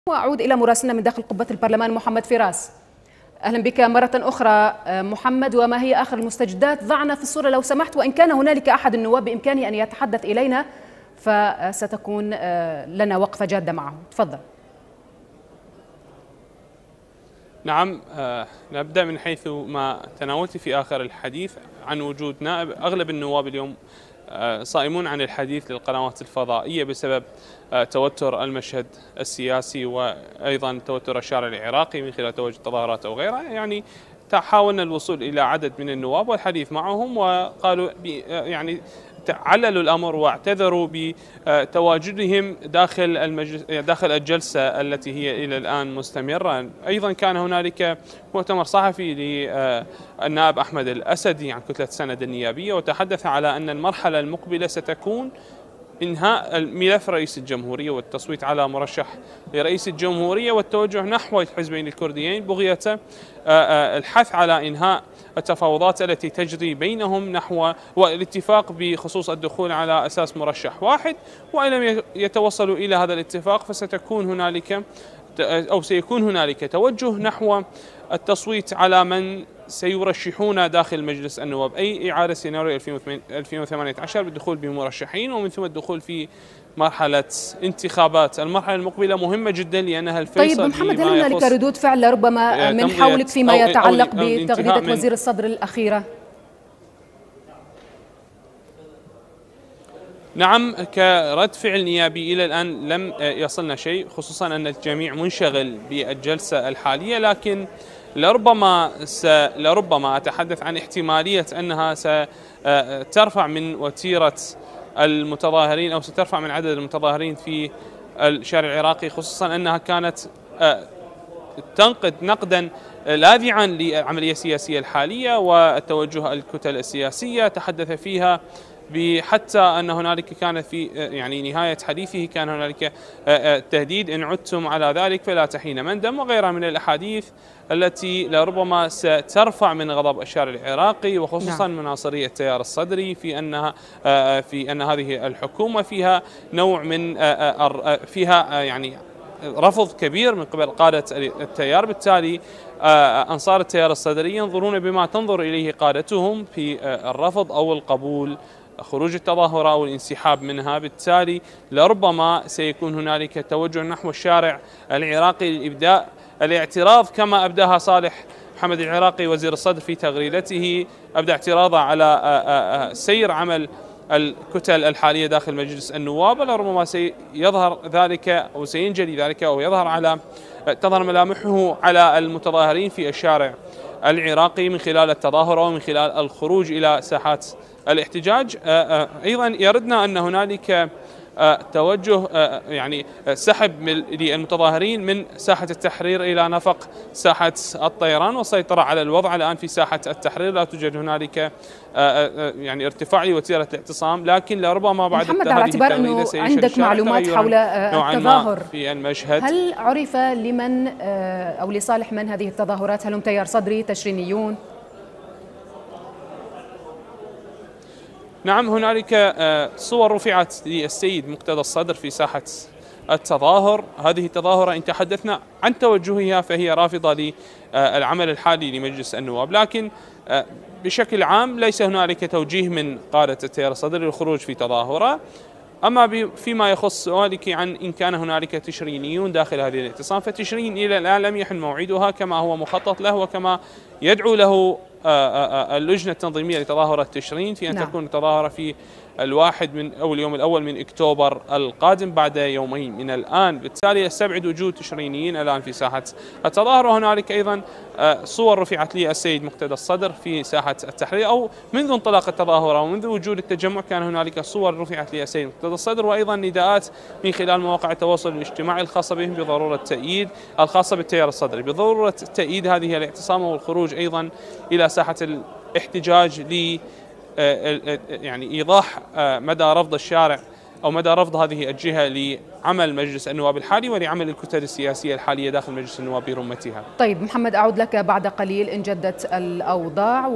اخر مستجدات جلسة البرلمان العراقي .. بعد تغريدة وزير الصدر
موقع قناة التغيير الفضائية الالكتروني